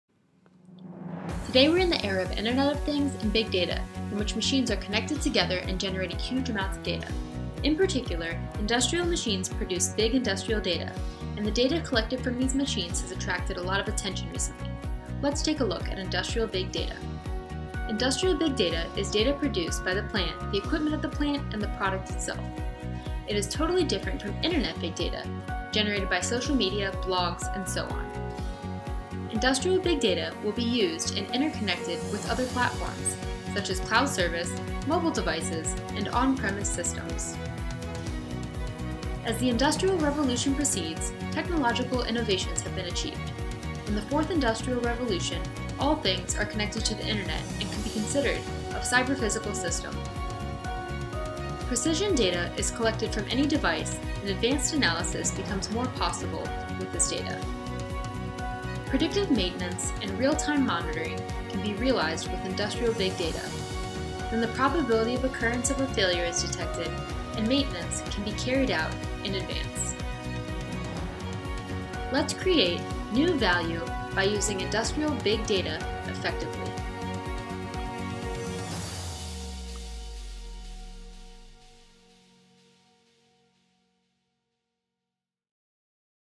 Industrial Big Data
Industrial Big Data brings you to Industrial Revolution 4.
In this video, we will talk what the Industrial Big Data is and how to use the data effectively.